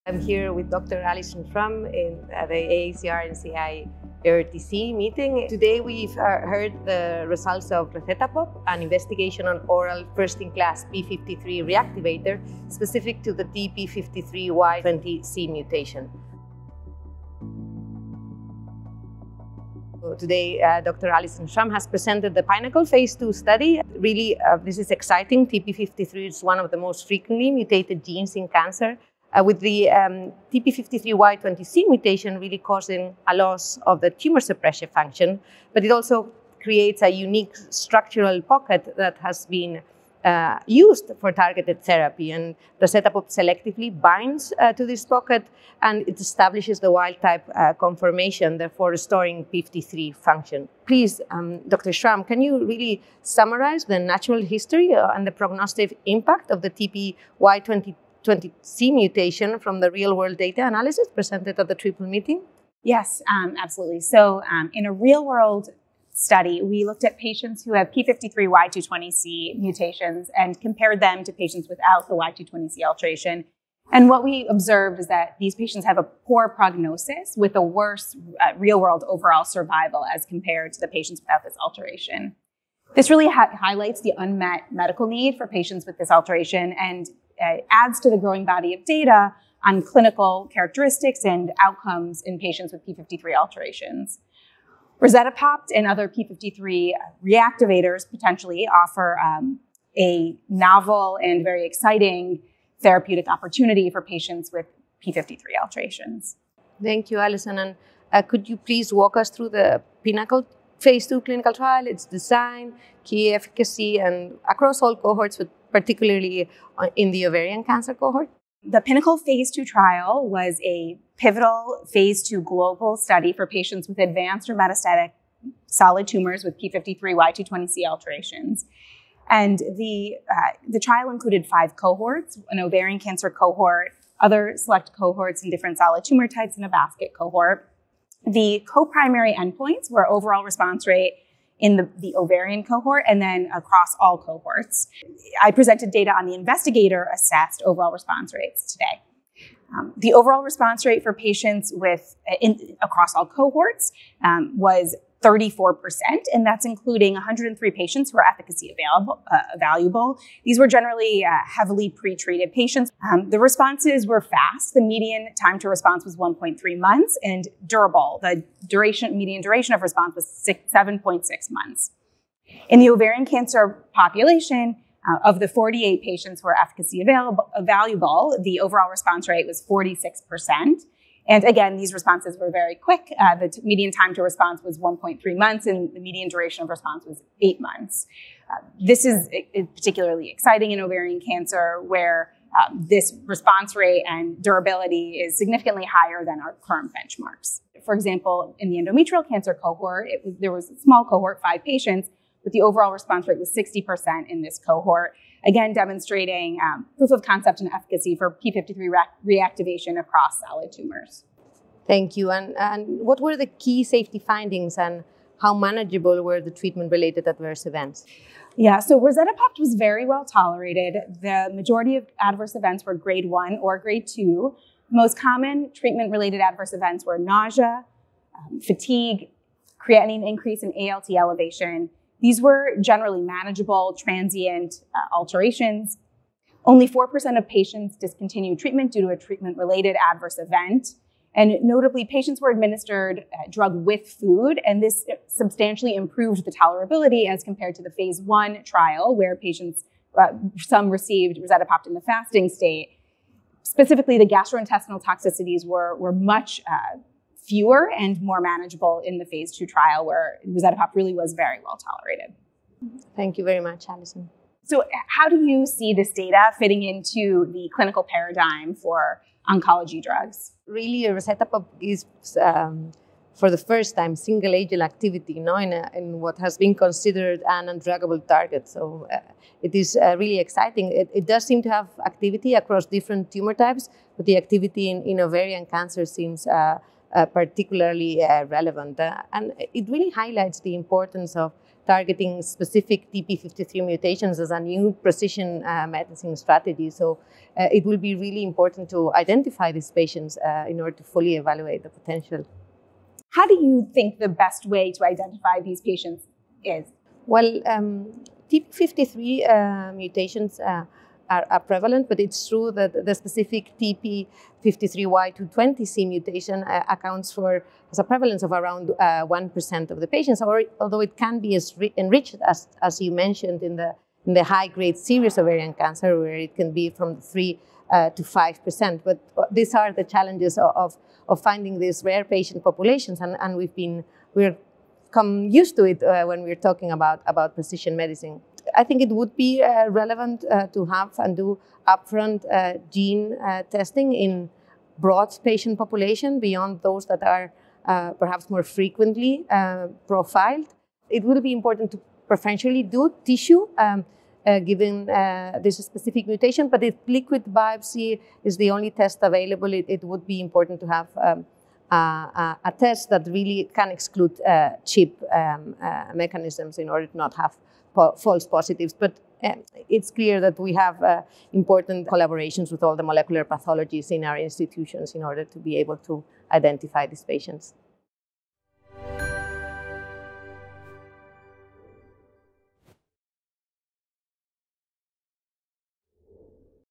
Update From AACR-NCI-EORTC: PYNNACLE phase 2 study
Drs Alison Schram and Elena Garralda share expert insights on the PYNNACLE Phase 2 initial analysis at AACR–NCI–EORTC 2025.

The PYNNACLE phase 2 study evaluates rezatapopt, a first-in-class p53 reactivator, as a treatment for TP53 Y220C–mutant advanced solid tumours.

In this video, the experts discuss:

• TP53 Y220C mutation and the rationale for mutant p53 reactivation
• Study overview and practice implications, spotlighting ovarian cancer
• Testing strategies for early and accurate patient identification

*Rezatapopt is an investigational agent which is not approved by a regulatory agency

Clinical takeaways:

• Reactivating p53 Y220C protein is a promising therapeutic strategy for patients with solid tumours specifically ovarian cancer, with TP53 Y220C mutations, where effective targeted therapies and biomarkers are limited.

• Rezatapopt is a first-in-class investigational p53 reactivator that selectively binds to the mutated p53 Y220C protein, stabilising its structure to restore wild-type tumour suppressor activity.

• Rezatapopt demonstrates rapid, durable and broad antitumour activity in TP53 Y220C–mutant solid tumours, with a particularly strong signal in ovarian cancer, and a generally manageable safety profile.

• Looking ahead, it will be important to prioritise early and accurate identification through up-front comprehensive genomic profiling using NGS with TP53 gene coverage. Tissue is preferred; when tissue is limited, selected ctDNA assays capable of excluding CHIP may be used.

Published October 2025